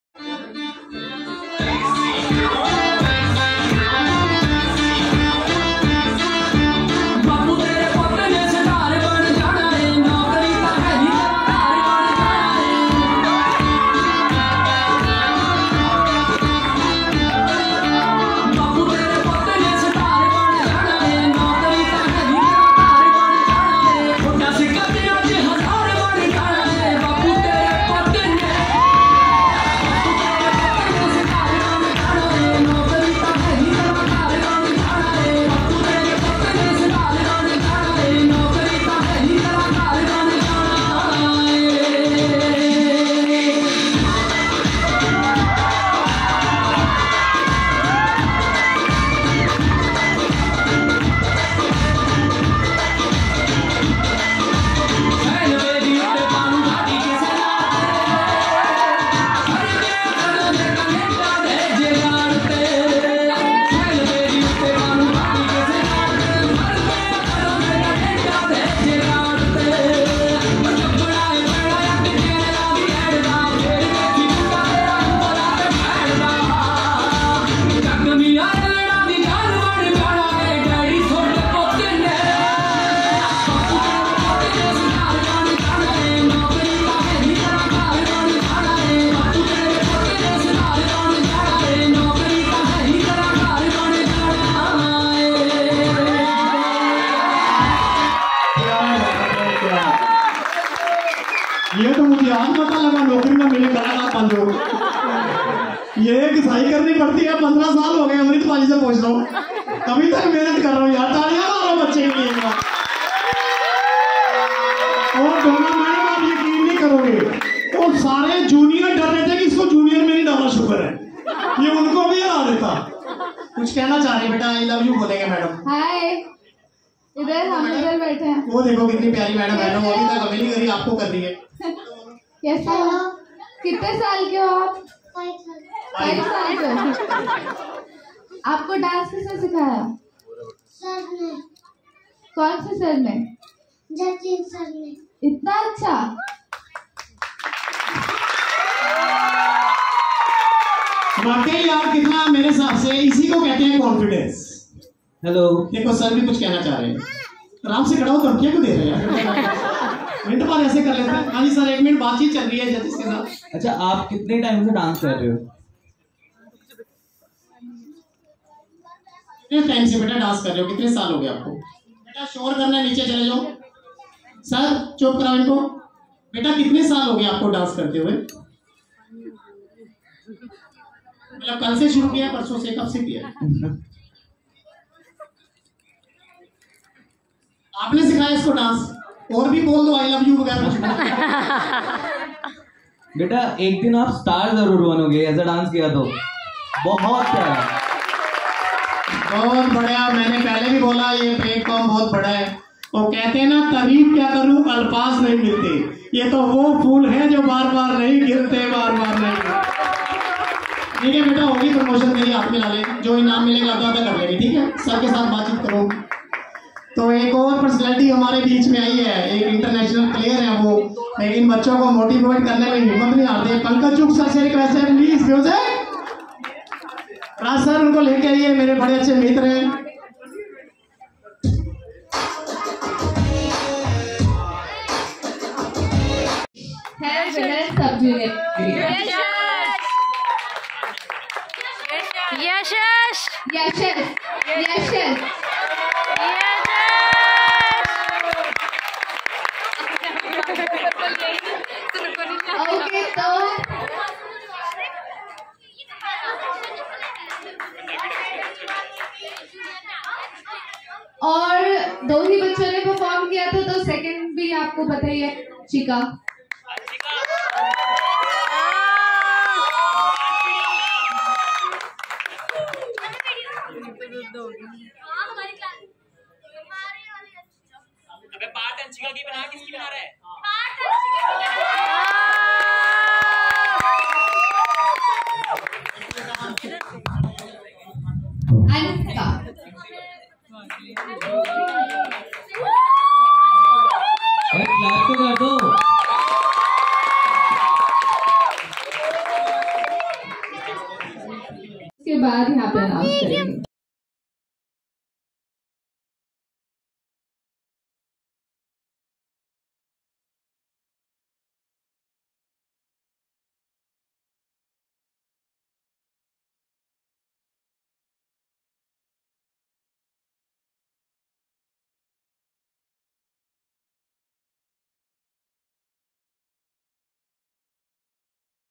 yashas ne jite prize kaise dekhte hai vedio main
daance performance DAV college yamuna nagar judge ms duna and ashishchawria